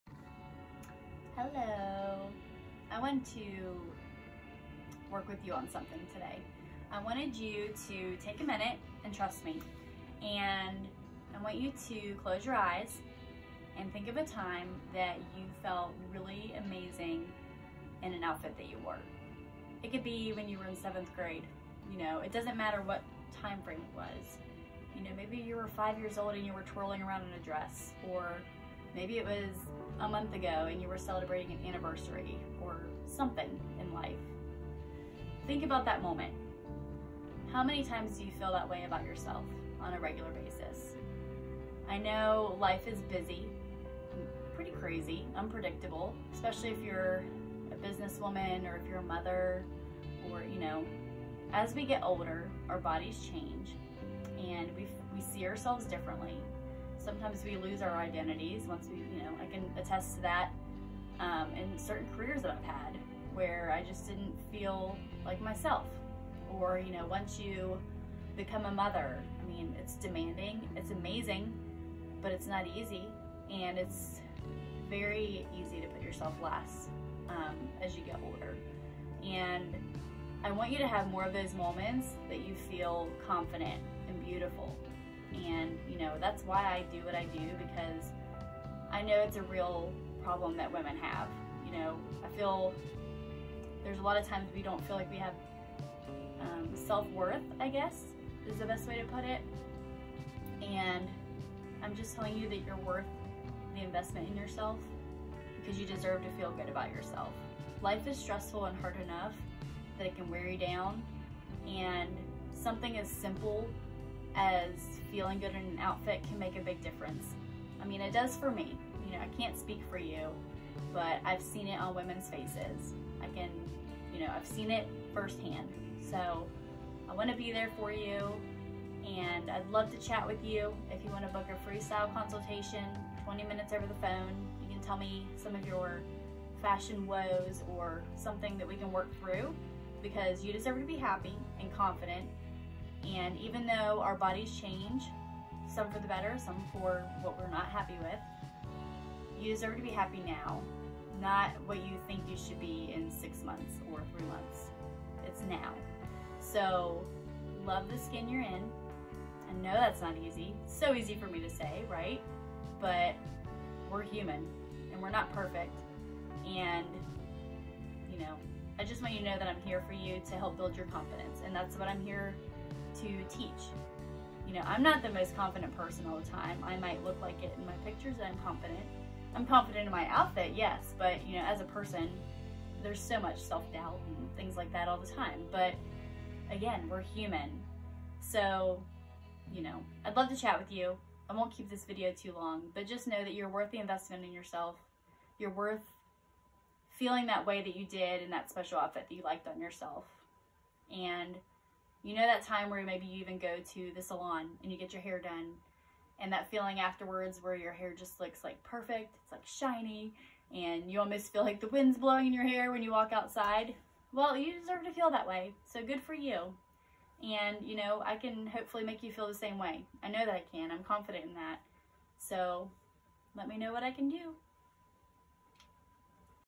Invest in Yourself
Let’s talk self-worth. Life is hard and stressful, so imagine a time when it wasn’t. Ladies, let’s get you back to the place where you took even a minute for yourself. You deserve to feel GOOD and CONFIDENT.